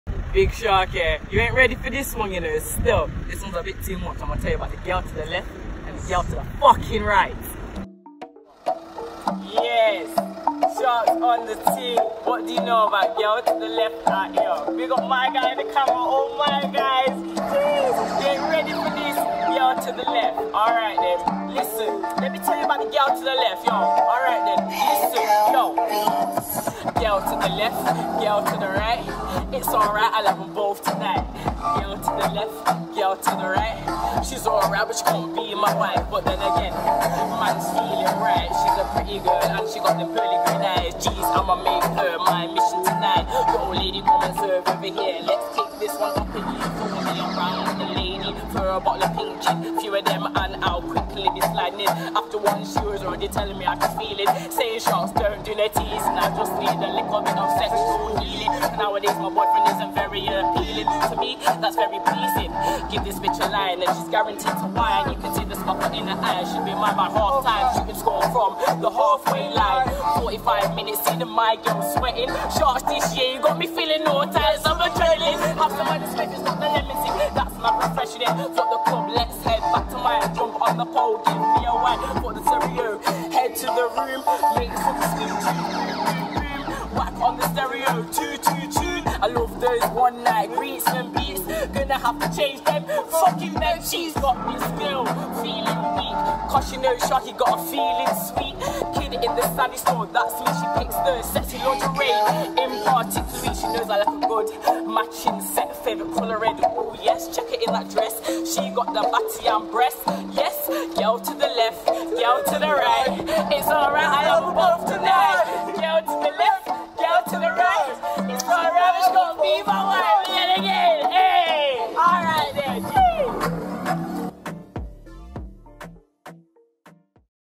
BarsBeatsTv - Sharky [BarzOnRoad]
Sharky from Nottingham drop us a fire BarzOnRoad first female artist to be on the echannel